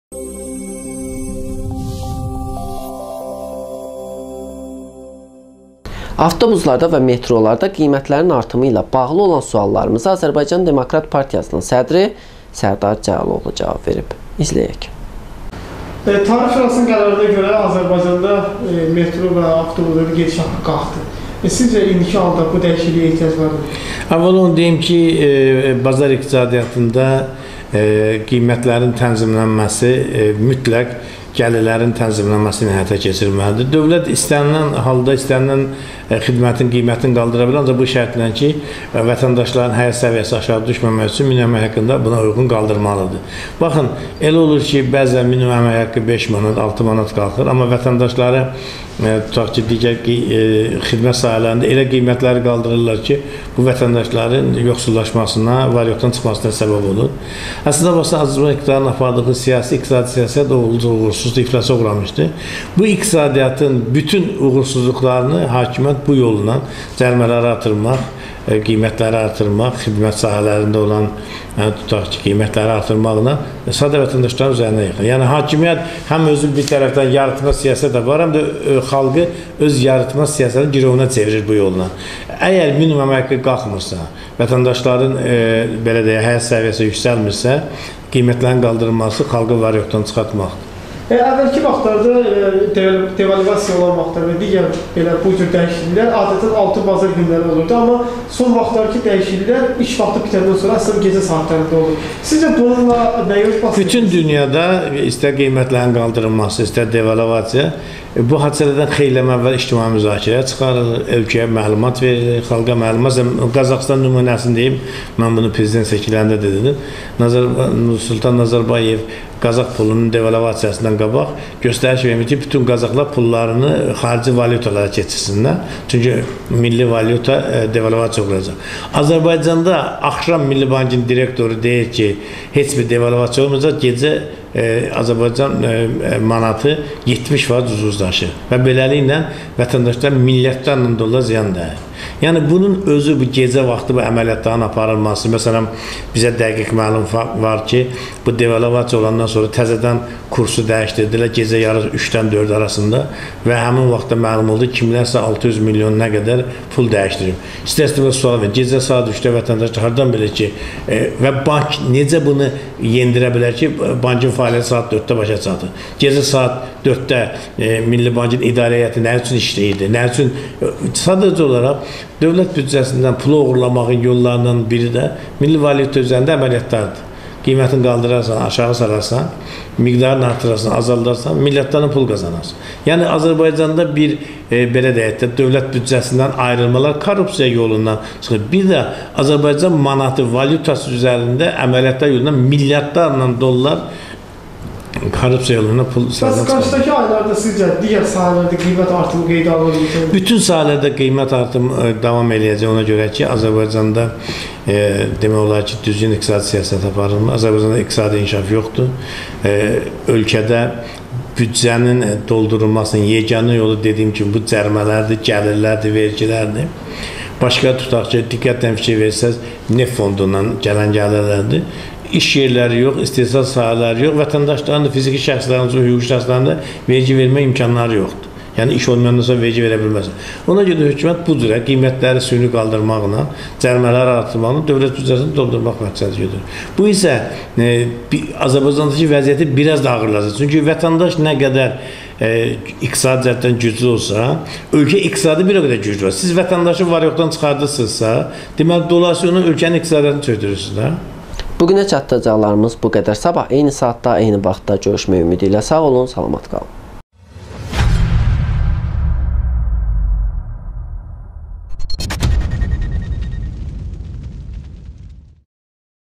"Qiymətlər qalxanda maaşların qalxmaması xalqın var-yoxdan çıxmasına səbəb olur "-Sərdar Cəlaloğlu
Azərbaycan Demokrat Partiyasının sədri Sərdar Cəlaloğlu:"Dövlət istənilən halda qiymət artımı edə bilər, ancaq vətəndaşların əmək haqqı da bna uyğun olaraq qaldırılmalıdır. Ancaq qiymətləri elə qaldırırlar ki, bu zaman vətəndaşlar var-yoxdan çıxır. Azərbaycanın apardığı siyasət olduqca uğursuzdur. Hakimiyyət yarıtmaz siyasət aparır və vətəndaşlardan da istifadə edir.